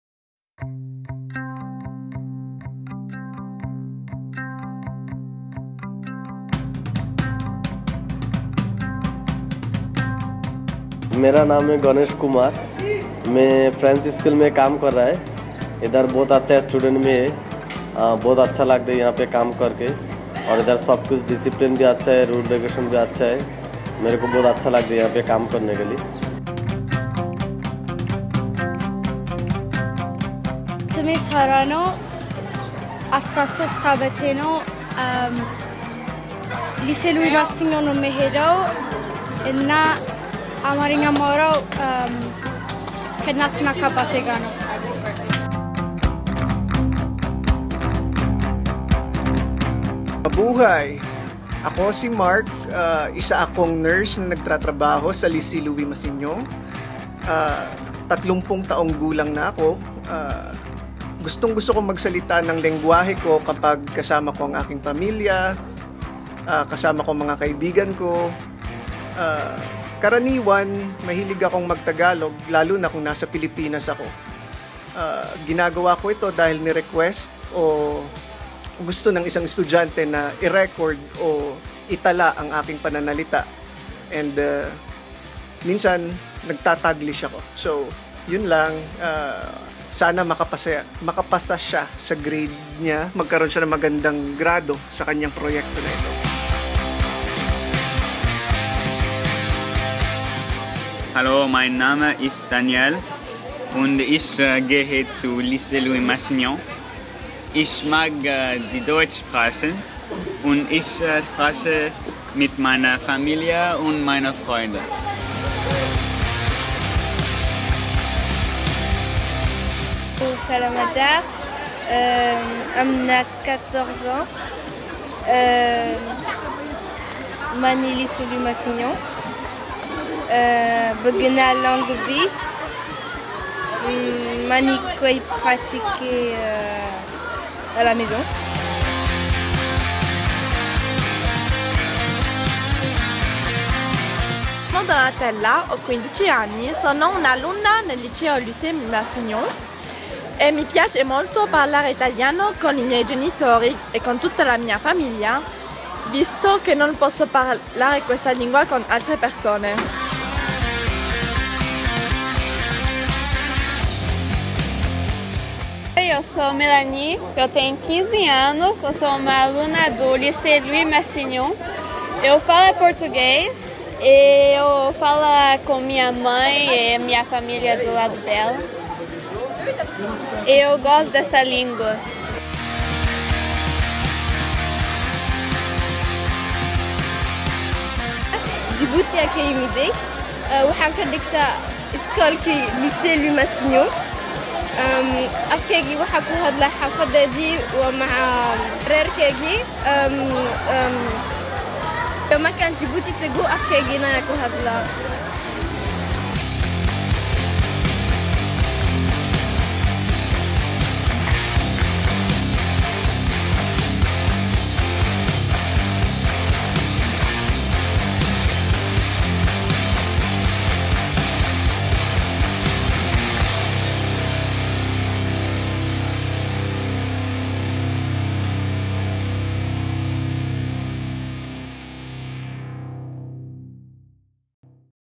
Lenguas en Louis massignon
Varias personas, estudiantes y empleados que trabajan en el Lycée Louis Massignon de Abu Dhabi (UAE) se presentan en su lengua primera.